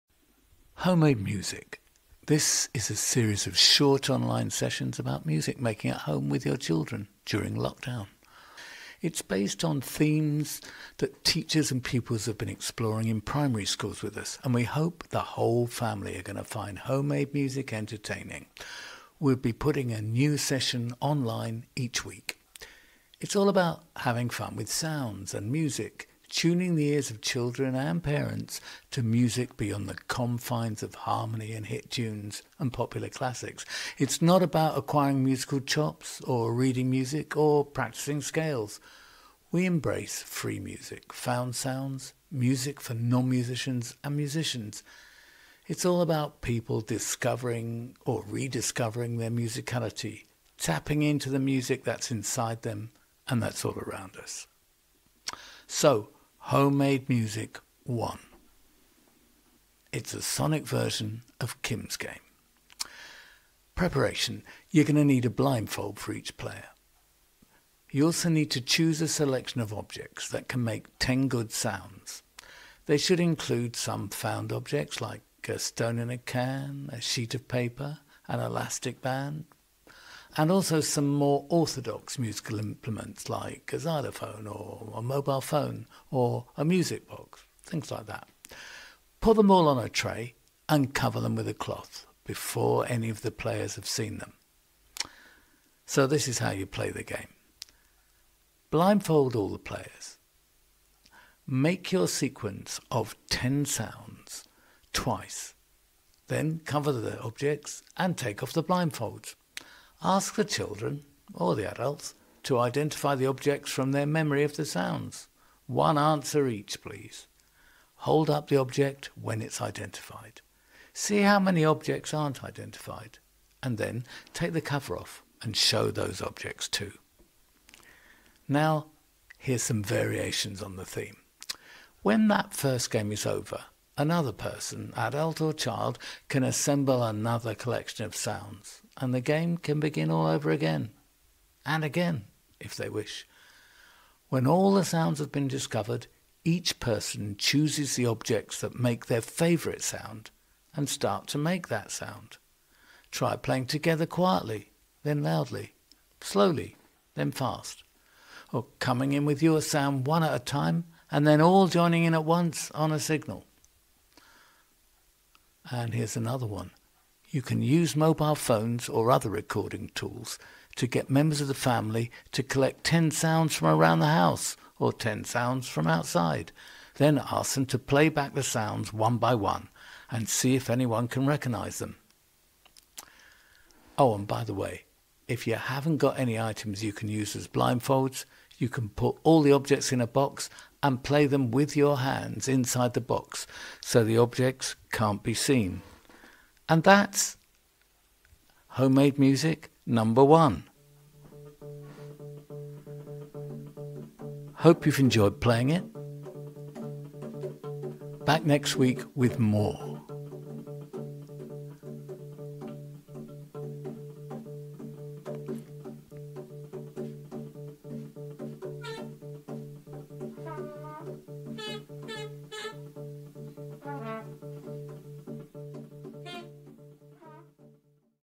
Home Made Music #1 | Narrated by Max Reinhardt | Oxford Contemporary Music (OCM)
Max Reinhardt has created a series of short online sessions to help you make music at home with your children during lockdown. We hope the whole family will find Home Made Music entertaining.

It's all about having fun with sounds and music, embracing free music and found sounds, rediscovering your musicality and tapping into the music inside you and all around us.

The sessions are for everyone, whether you've got music making experience or not and don't worry, you don't need musical instruments - we'll show you the household objects you can use for the sessions.

-Transcript of this video is available as a pdf download